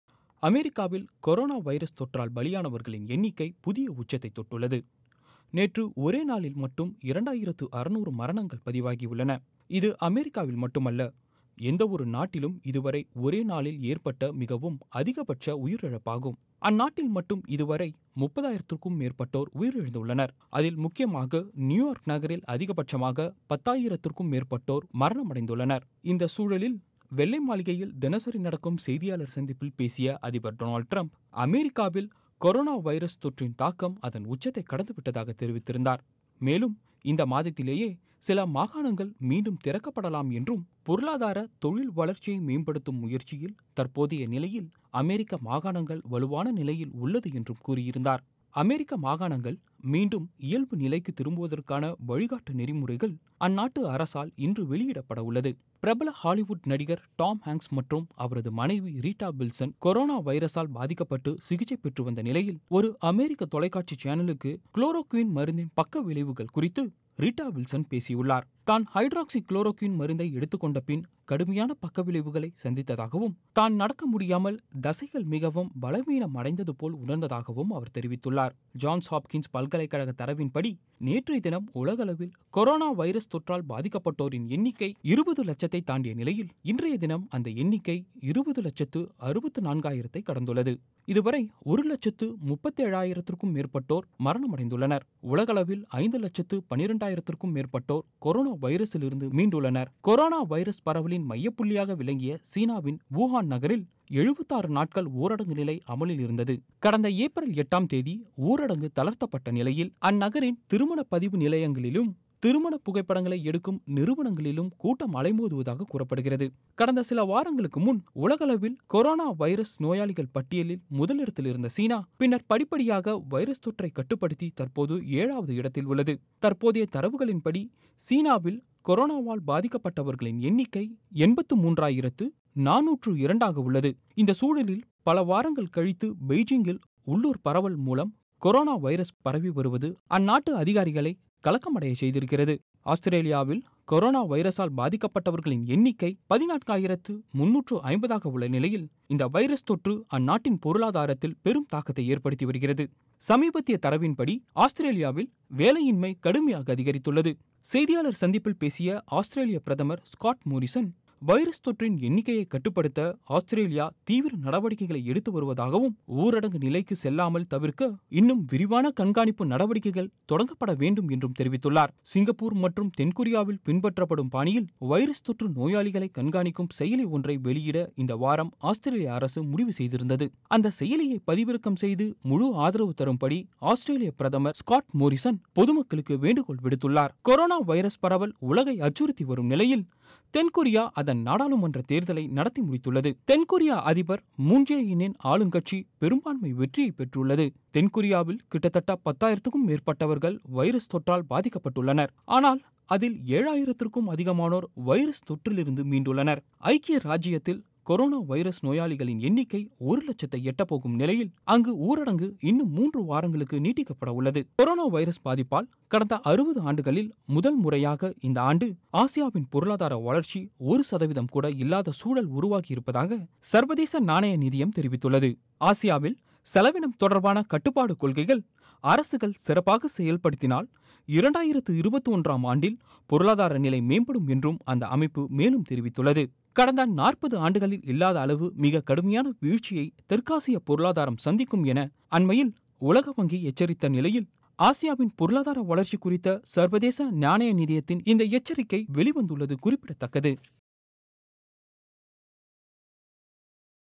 Corona Virus News - ஒரே நாளில் 2600 பேர் பலி - World update on கொரோனா வைரஸ் America, UK, Australia
அமெரிக்காவில் கொரோனா வைரஸ் தொற்றால் பலியானவர்களின் எண்ணிக்கை புதிய உச்சத்தை தொட்டுள்ளது.

நேற்று ஒரே நாளில் மட்டும் 2600 மரணங்கள் பதிவாகியுள்ளன. இது அமெரிக்காவில் மட்டுமல்ல எந்த ஒரு நாட்டிலும், இதுவரை ஒரே நாளில் ஏற்பட்ட மிகவும் அதிகபட்ச உயிரிழப்பாகும்.

அந்நாட்டில் மட்டும் இதுவரை 30,925 பேர் உயிரிழந்துள்ளனர். அதில், முக்கியமாக நியுயார்க் நகரில் அதிகபட்சமாக 10,899 பேர் உயிரிழந்துள்ளனர்.